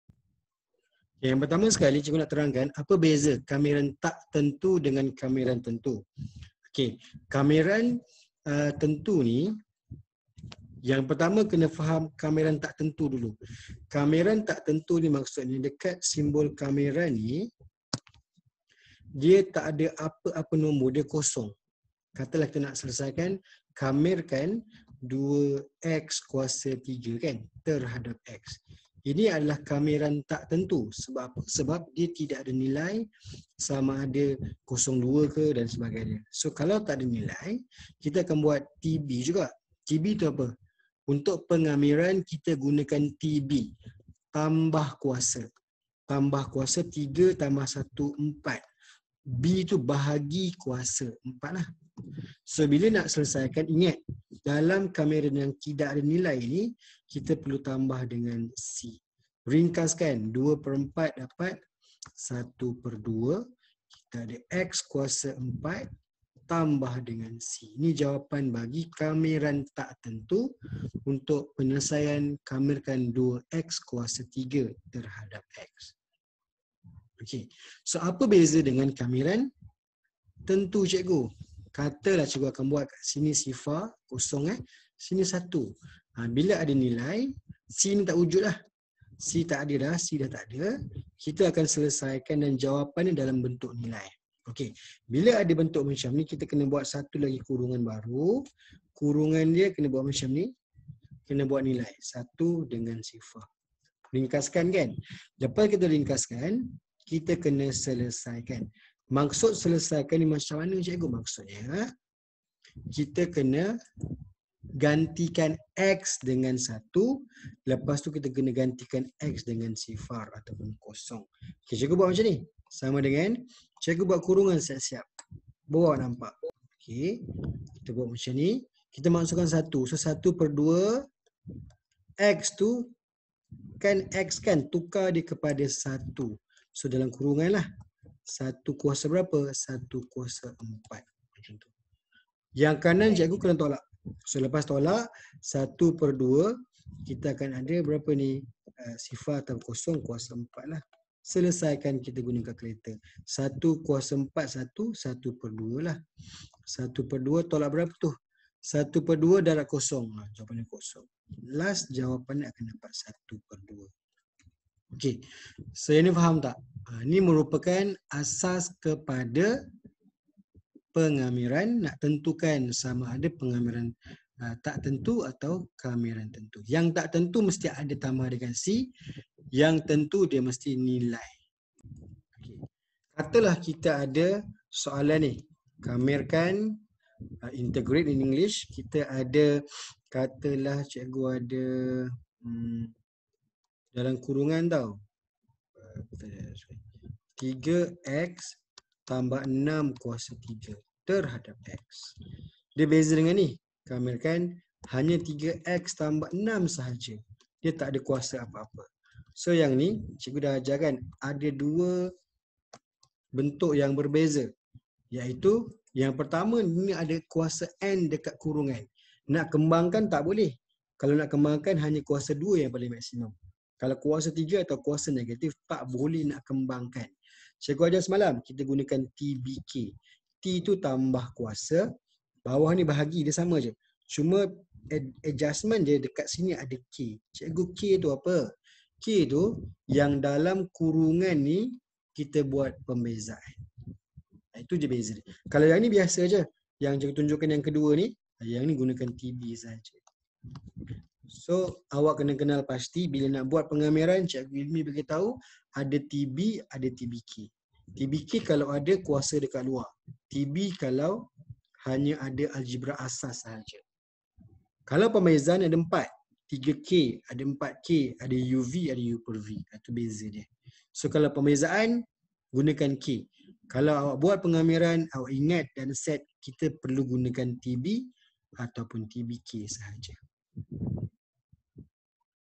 BEZA KAMIRAN TAK TENTU DAN KAMIRAN TENTU
Penerangan ini membantu pelajar faham perbezaan antara Kamiran Tak Tentu Dan Kamiran Tentu